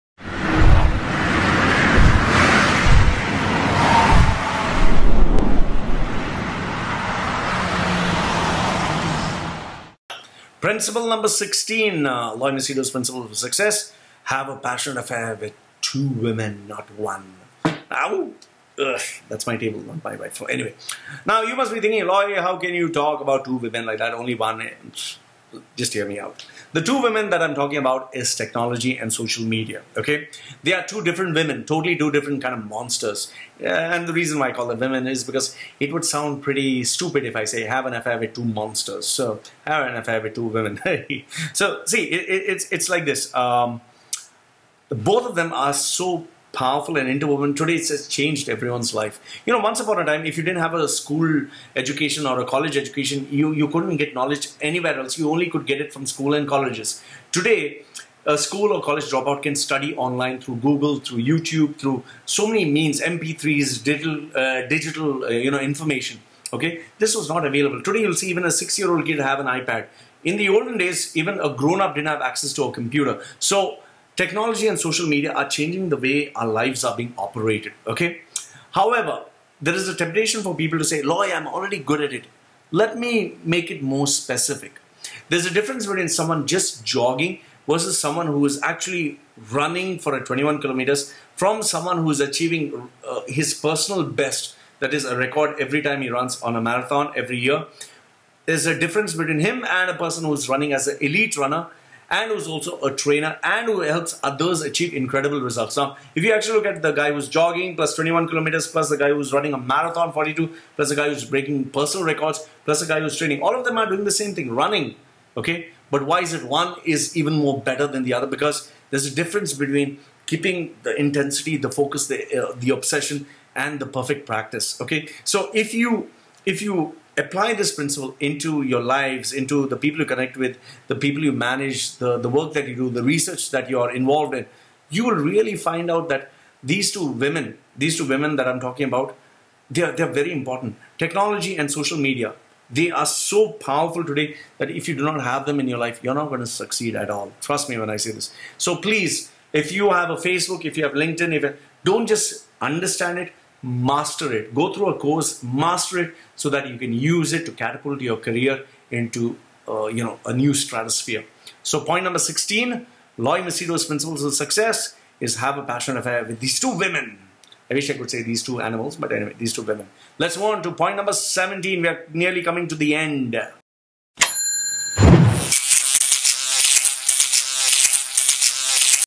Loy Machedo's 21 Principles Of Success - Number 16
HAVE A PASSIONATE AFFAIR WITH THESE TWO   WOMEN
Behind every successful man – are these two women
1) Technology
These two women keep evolving,
giving birth to new babies and
constantly out the attention, money and time out of its users.
Now you get tempted to assume
After all you keep posting, loading and sharing
Correct?
To give you an analogy it is more like
Someone jogging everyday
Versus
Someone running a 21kms race
Versus
Someone who keep achieving a Personal Record while running a Marathon
Versus
Someone who is respected as among the Top Elite
At Running a Marathon & also
Helping others improve and achieve incredible results.
Going through the   motions doesn’t make you an Industry expert.
Mastery is everything.
Keep this in mind
Social Media has changed the dynamics
How we do business
How we live our lives
How we connect with people
How we interact with people
How we manage our lives
How we educate ourselves
It completely changed everything
In fact – once upon a time if you didn’t go to college
You were finished
Today you do not need to go to college
Courtesy these two women.
So if you are not taming them
They are taming you.
Make sense?
Just keep this locked into your unconscious
Learn to tame these two women
You will be far ahead of your game
Light years ahead.